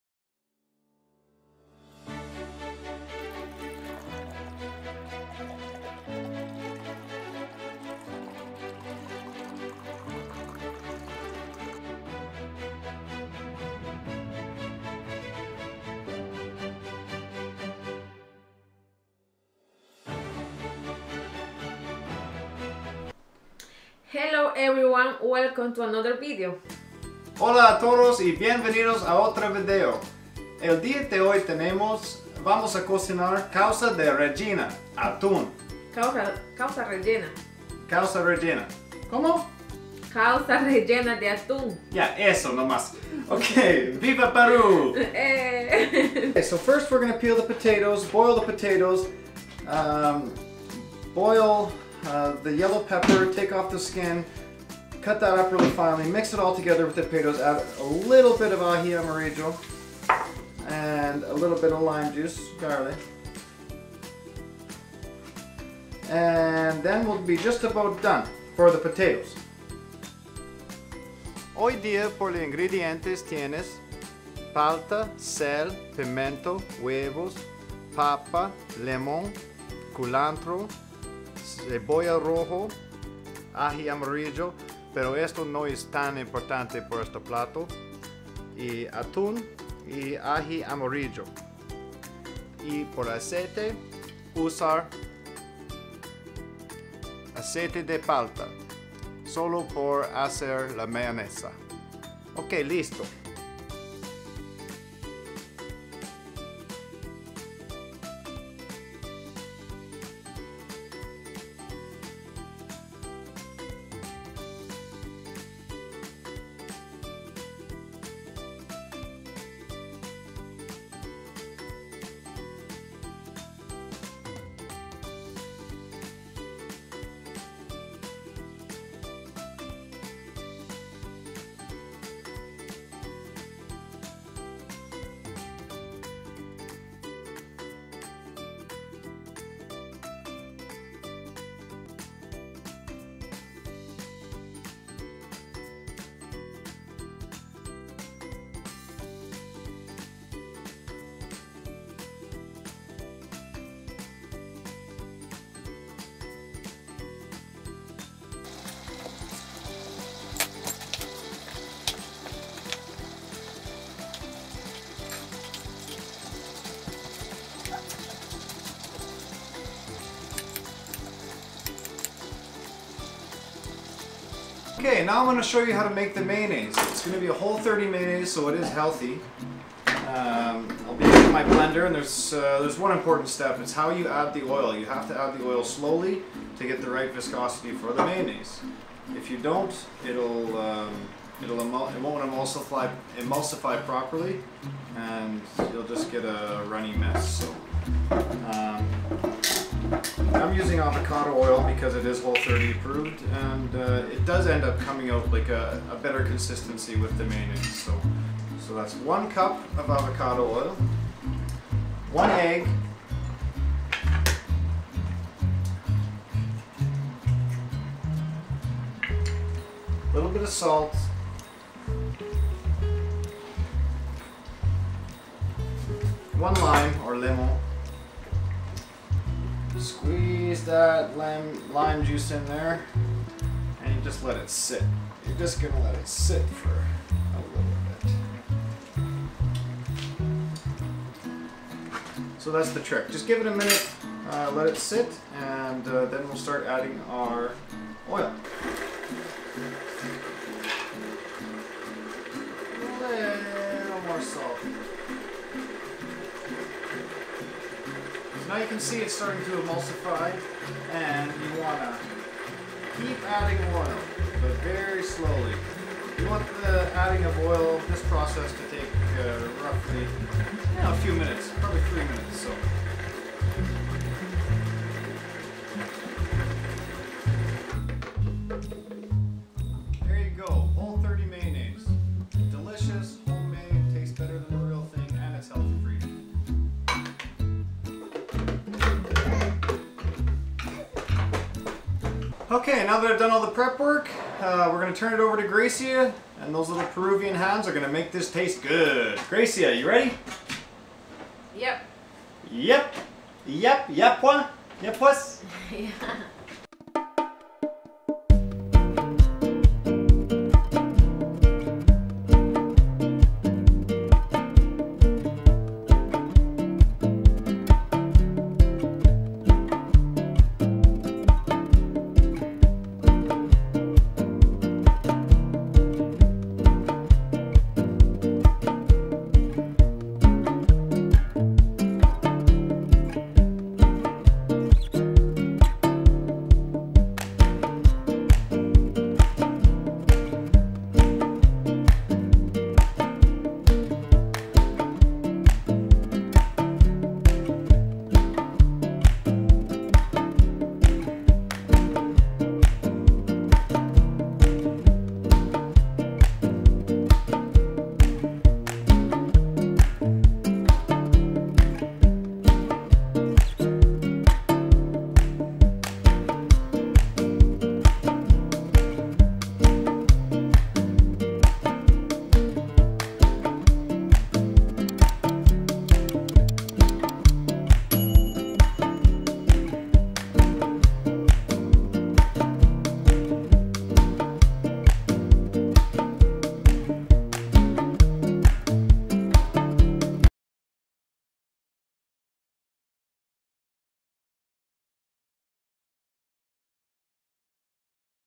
Cooking with Grecia - Causa Rellena
Causa takes its name from the old Incan Quechua word kausaq, which means “giver of life,” another name for the potato. Rellena is the Spanish word for “stuffed” or “filled.”

1) Boil and mash potatoes with a little aji amorillo
2) Mix in boiled yellow peppers after blending
3) Mix tuna with lime cilantro and finely diced red onion
4) Layer dish with potato mix then avocado then tuna mix then another layer of potato
5) Garnish with olive and cilantro

Causa toma su nombre de la antigua palabra quechua inca kausaq, que significa "dador de vida", otro nombre de la papa. Rellena es la palabra en español para "relleno" o "relleno".

1) Hervir y triturar papas con un poco de ají amorillo
2) Mezcle los pimientos amarillos hervidos después de mezclar
3) Mezcle el atún con el cilantro de lima y la cebolla morada finamente picada
4) Cubra el plato con mezcla de papa, luego aguacate, luego mezcla de atún y luego otra capa de papa
5) Decora con aceituna y cilantro